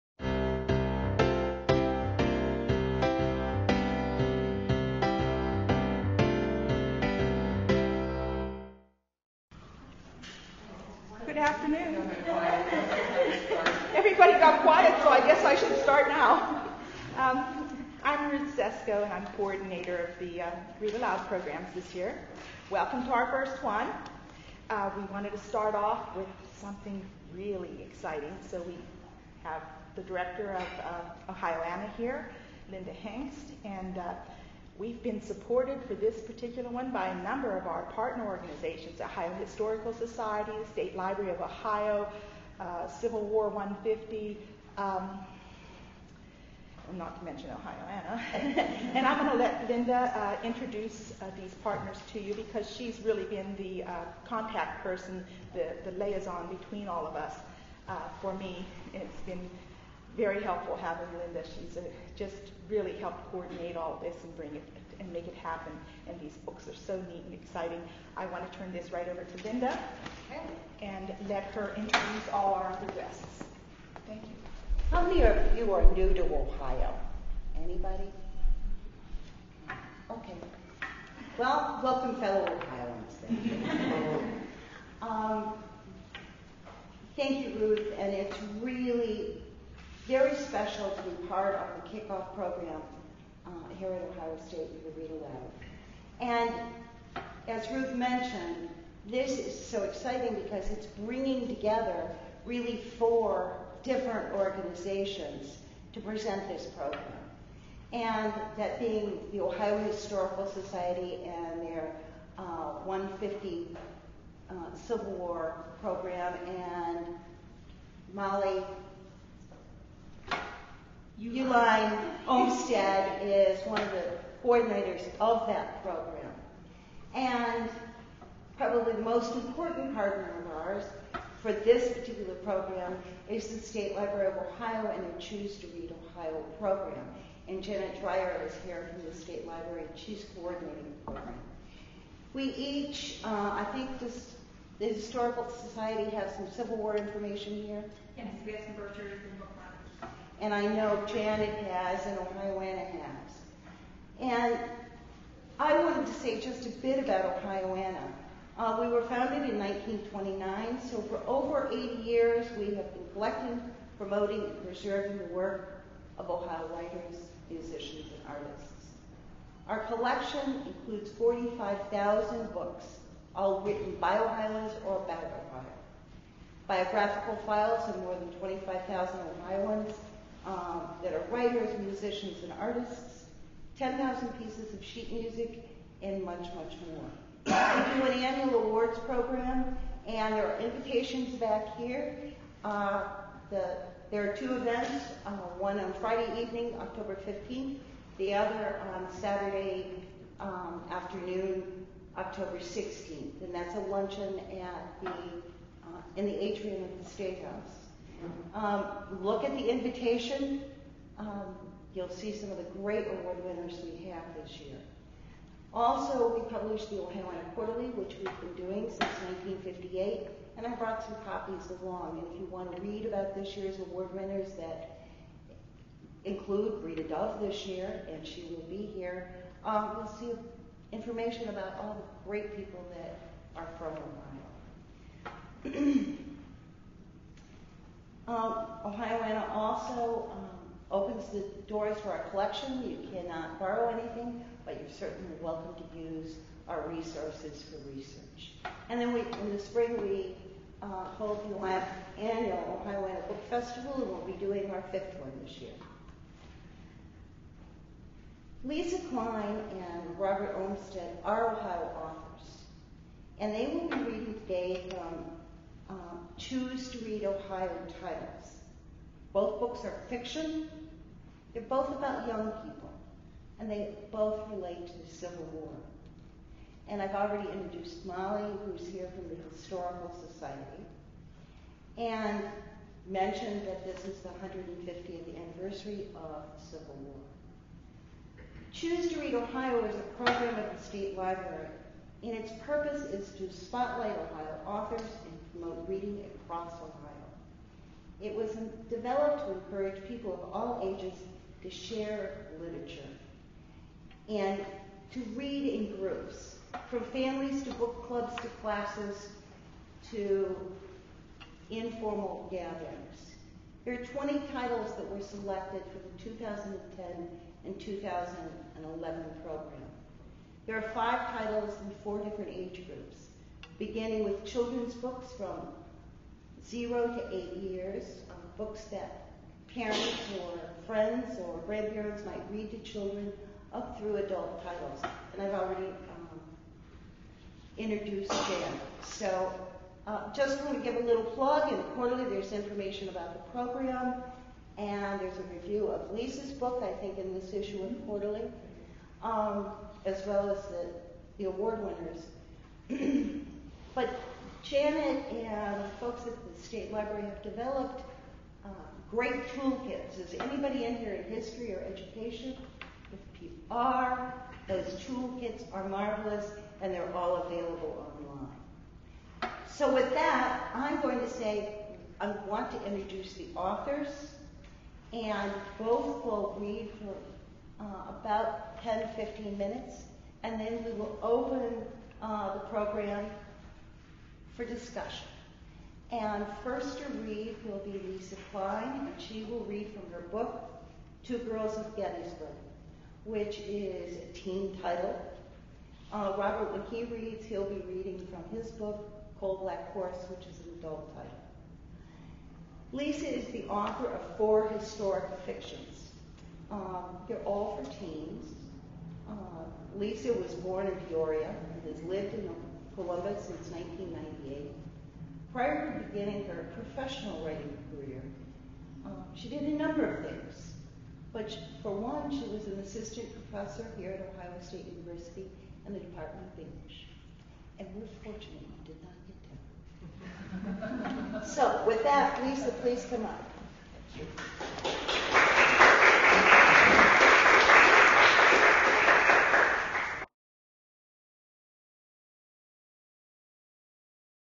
[ReadAloud at OSU] Two Ohioana Authors - Lisa Klein & Robert Olmstead
ReadAloud at The Ohio State University: September 30, 2010

Two Ohioana authors kicked-off the 2010-11 Read Aloud Program:  Lisa Klein read from Two Girls of Gettysburg and Robert Olmstead from Coal Black Horse . Both books deal with the effect of the Civil War on young people and both are 2011 Choose to Read Ohio titles. This event was supported by our partner organizations Ohioana, the State Library of Ohio, the Ohio Historical Society and Ohio Civil War 150.